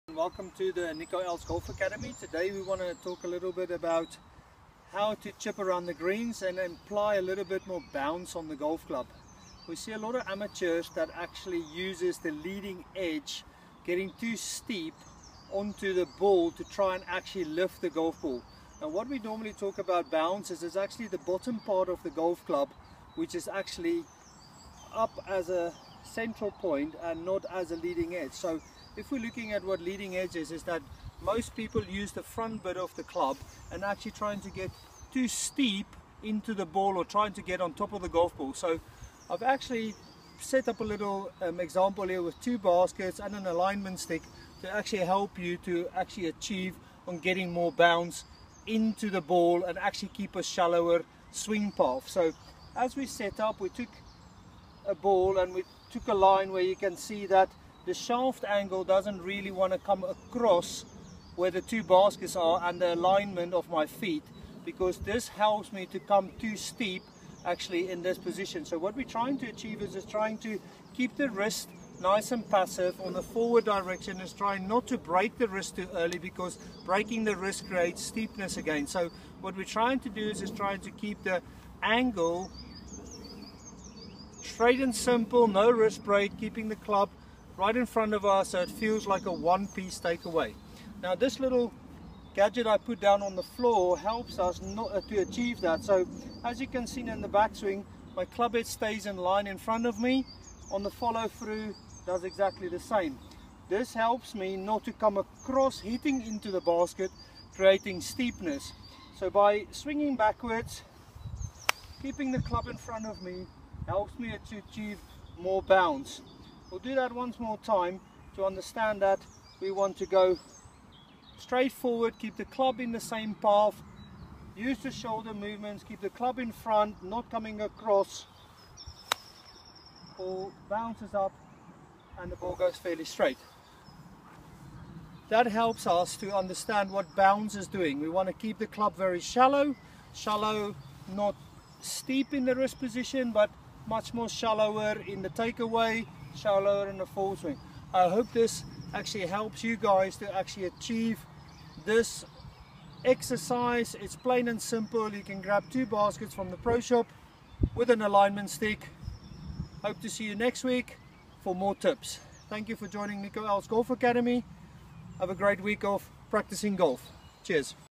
Nico Els Short game (chipping) lesson
Chipping with more bounce to control distance around greens